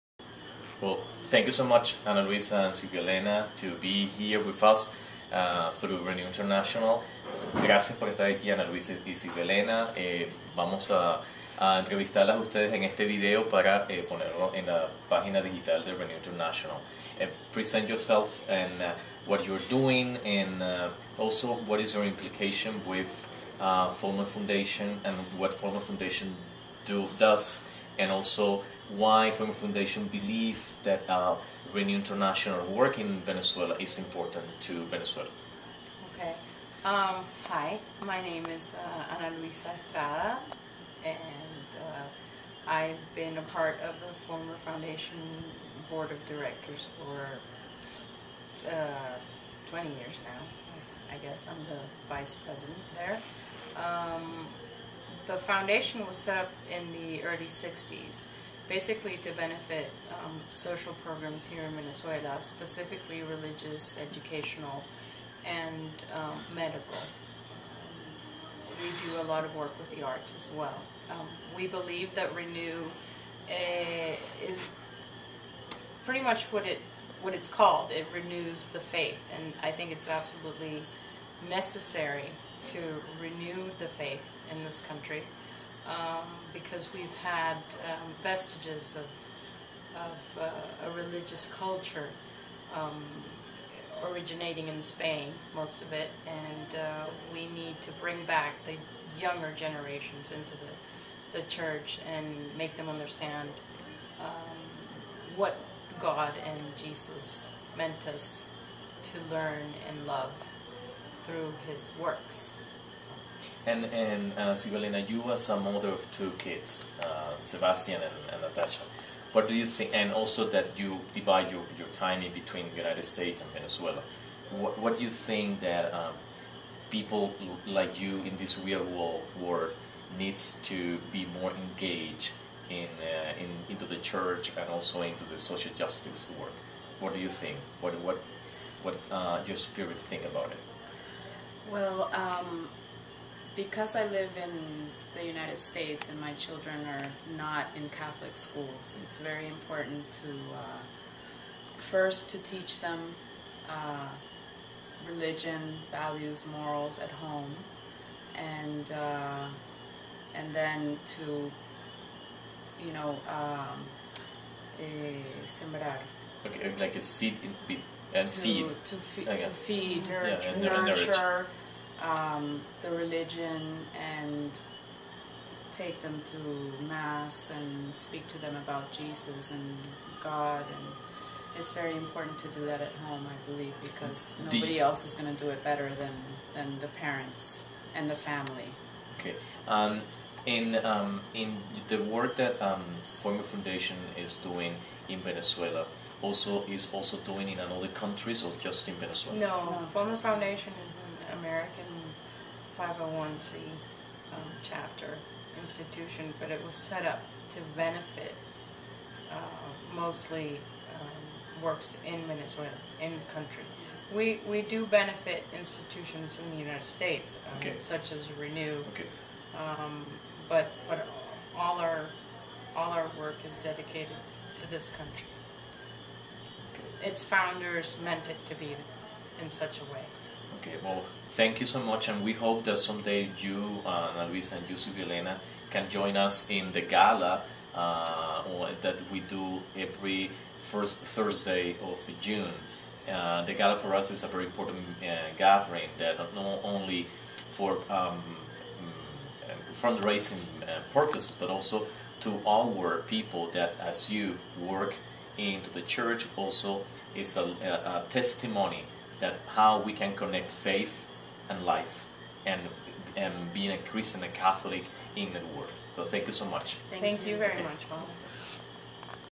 RENEW-ing the faith in Venezuela
Ana Luisa and Sylvia Elena Estrada from the Vollmer Foundation talk to Fr. Alejandro about why the work RENEW does is so important to renewing the faith in Venezuela.